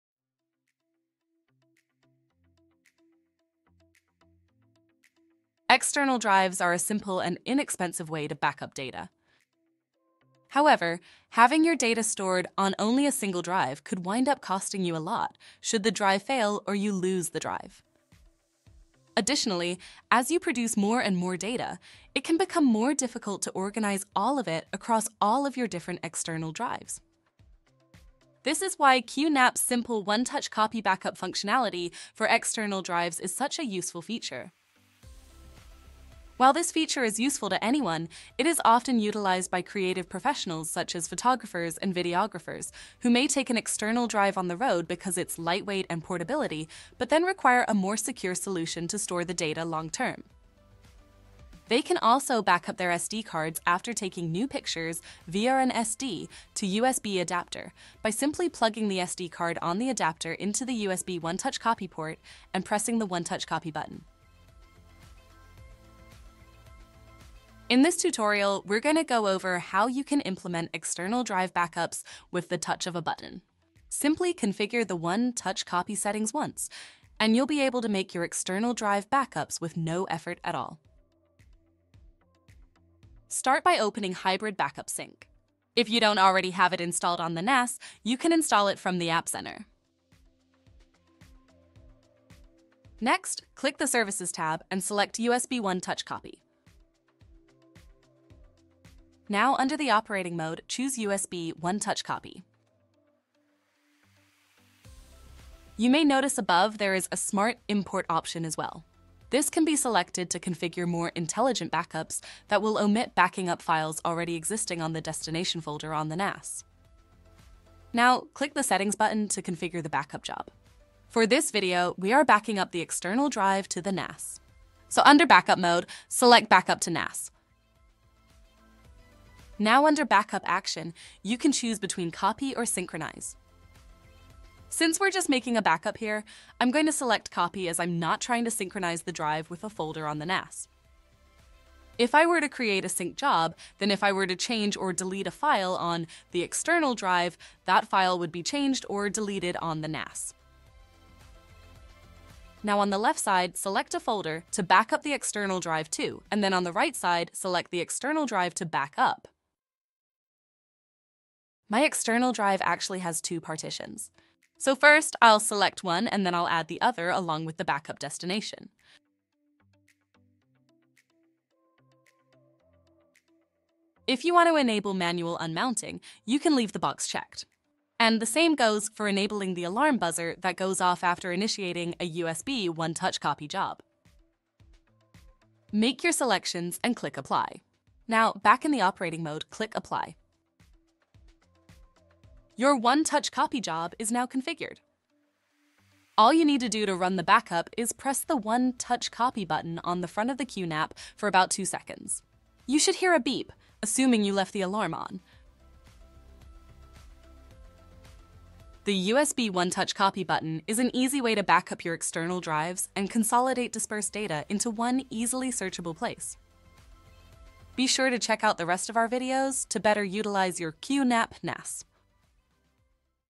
QNP331 Backup Strategies: External Drive Backup
Learn how to configure the USB One-Touch-Copy button in order to quickly and easily make backups to and from external drives with the touch of a button.